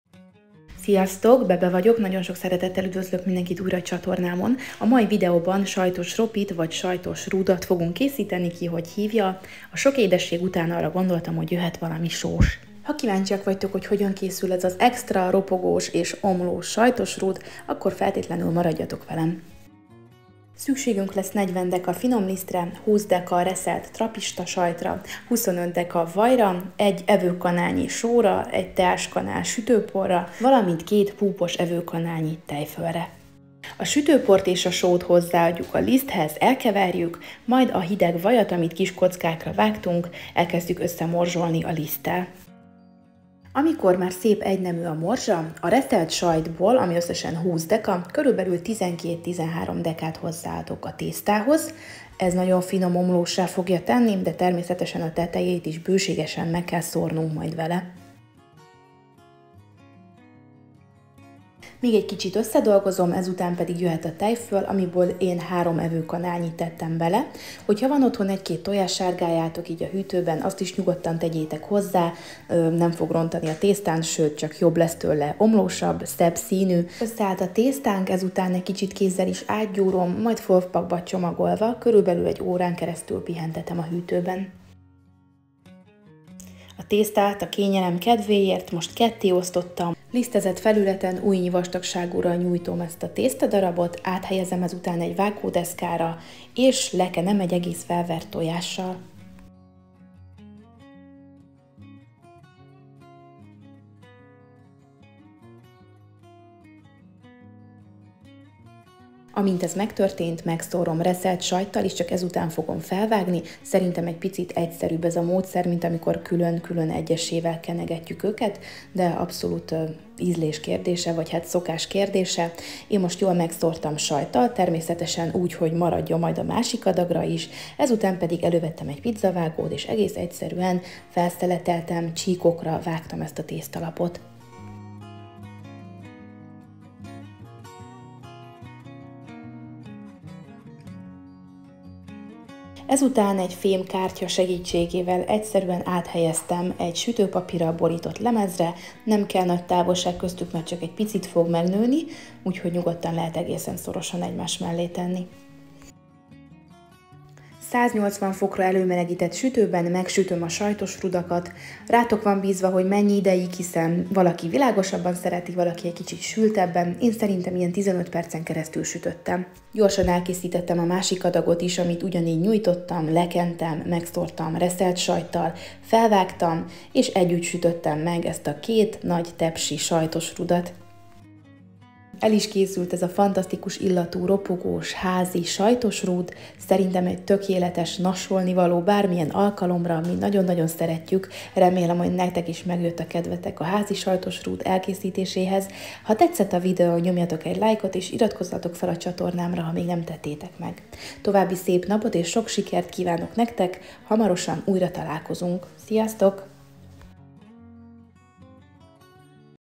SAJTOS RÚD🧀 TV ROPI - sós ropogtatnivaló - BebePiskóta
Sziasztok kedves barátaim,

 a mai videóban a sok édesség után végre egy kis sós ropogtatnivaló lesz a videó témája. Ha szeretitek a sós nasikat, vagy vendégeket vártok ez egy tökéletes választás lesz. Hamar elkészül, jó sok lesz belőle és garantáltan mindenkinek ízlelni fog 🤗fogadjátok szeretettel.

* 40 dkg Liszt
* 20 dkg trappista sajt
* 25 dkg vaj
* 2-3 ek tejföl
* 1 ek só
* 1 tk sütőpor
* 1 tojás a lekenéshez

Sütés 180 fokon 15 perc 🔥